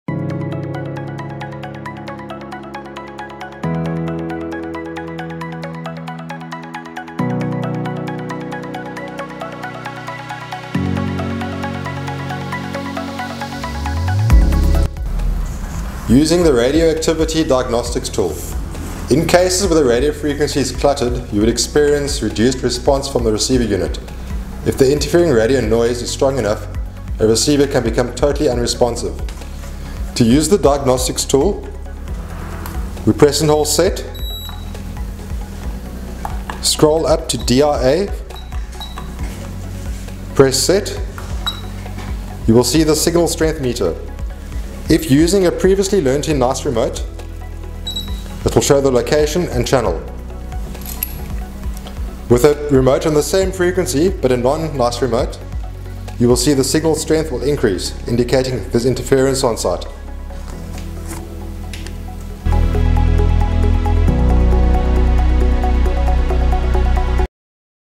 Nice Receiver: RX3 / Condo - Radio Frequency Diagnostics
The Nice RX3 / Condo has a radio frequency interference diagnostics tool, here is a Nice video to take you through the steps of using it.